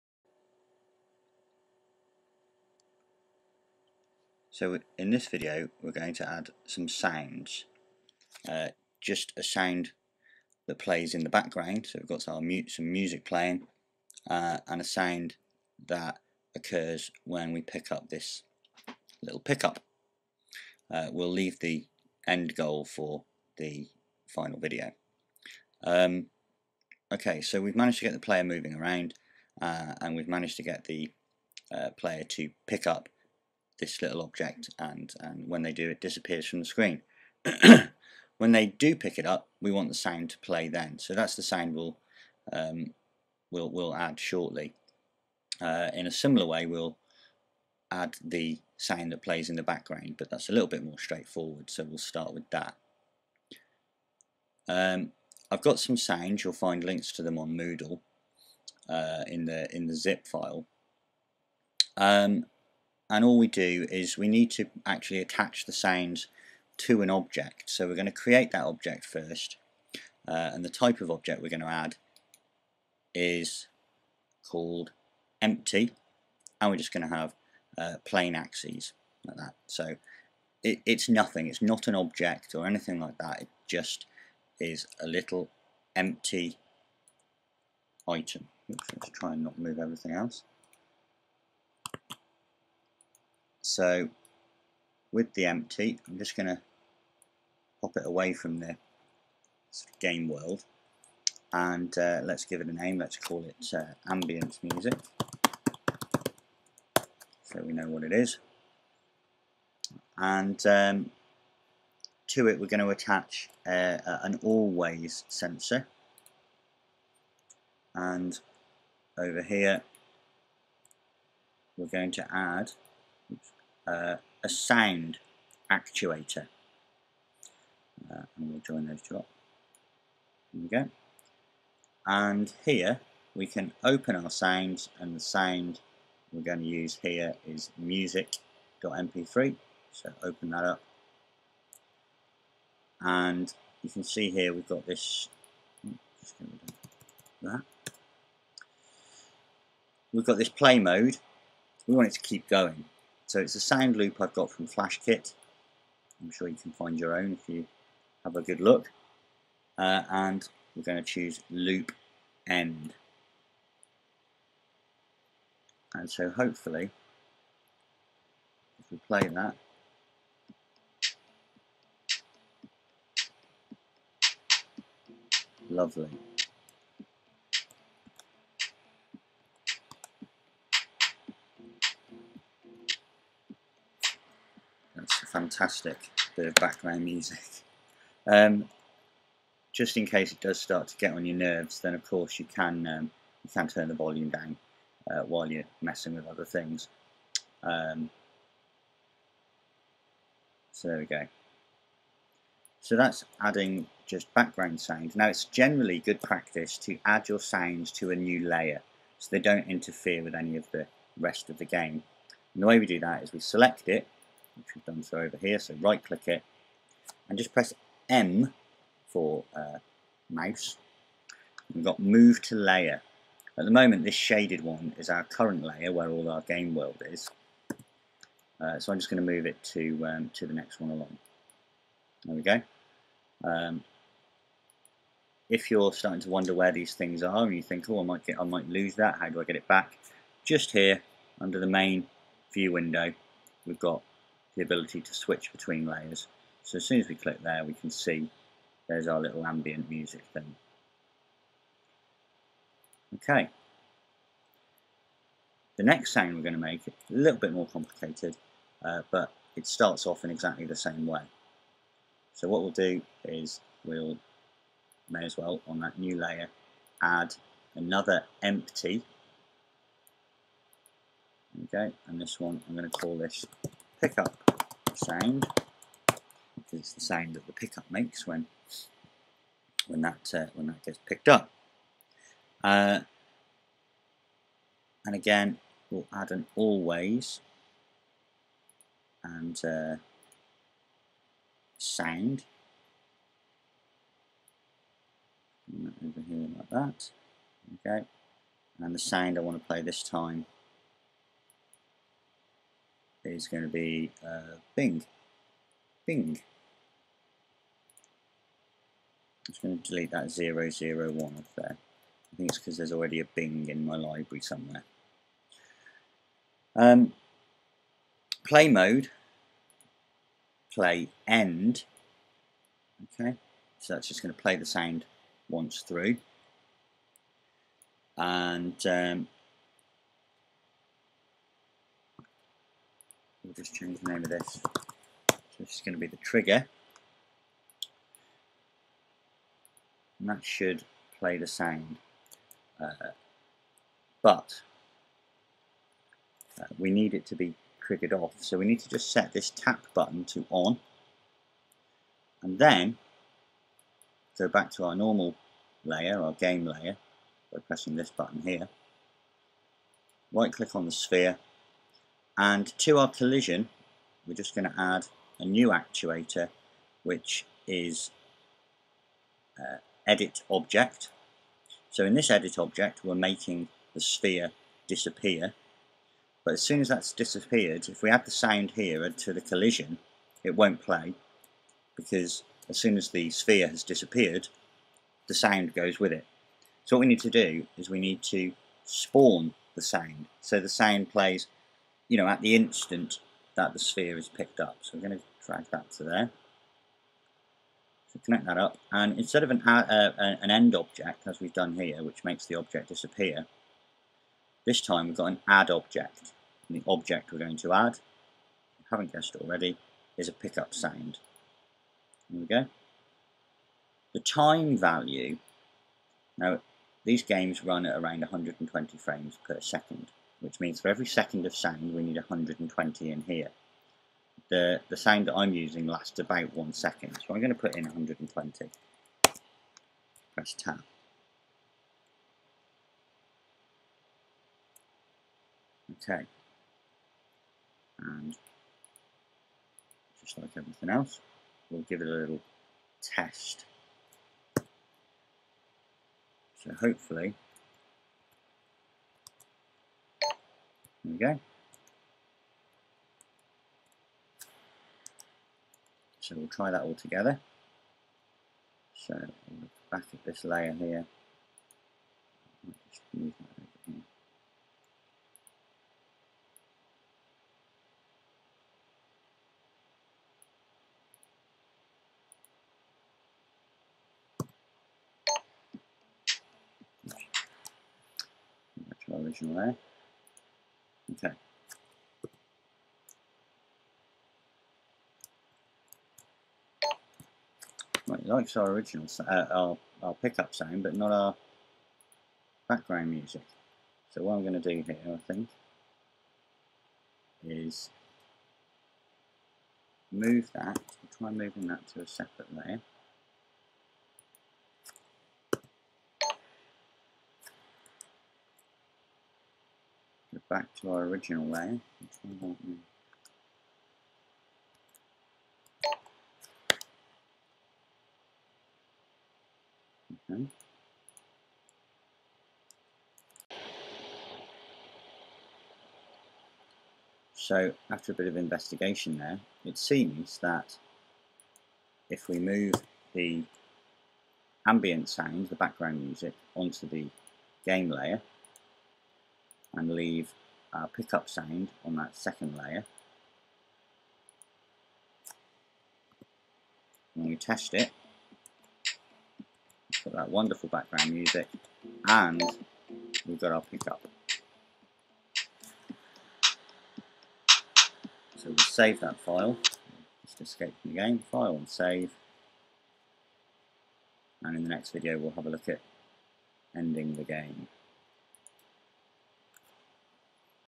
Blender Game Engine for Beginners: Part 4 - Sounds & Music
This video shows you how to add sounds such as background music or sound effects to your game.

so feel free to find your own